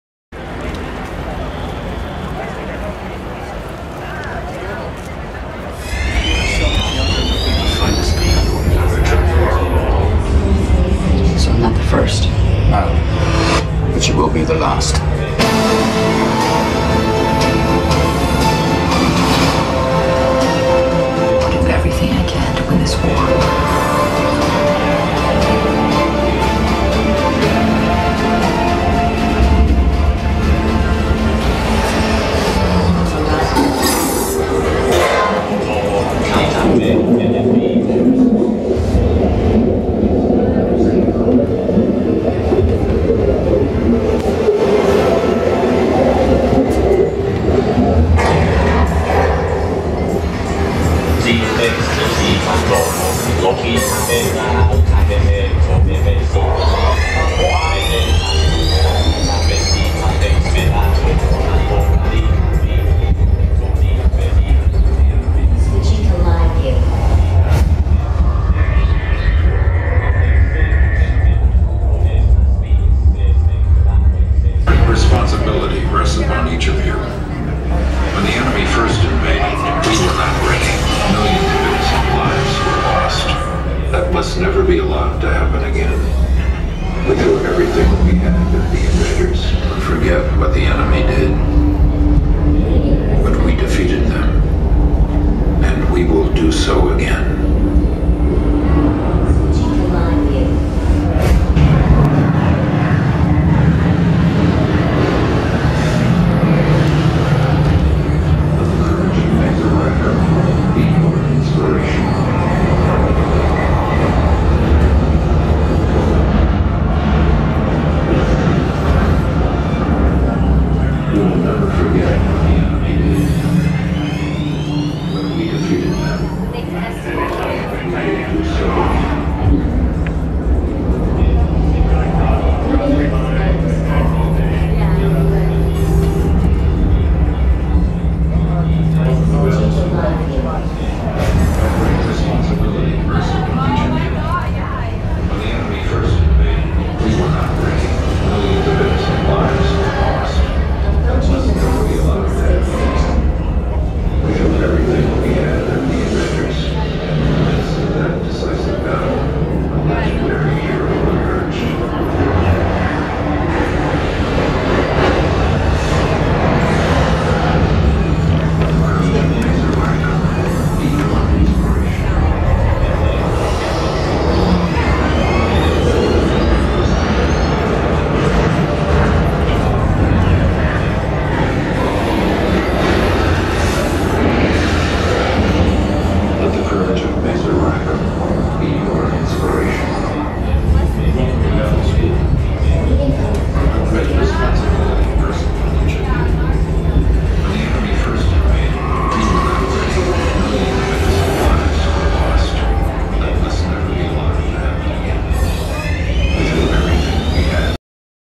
Ender's Game - SDCC Fan Experience
A walk through the Ender's Game Fan Experience at San Diego Comic Con.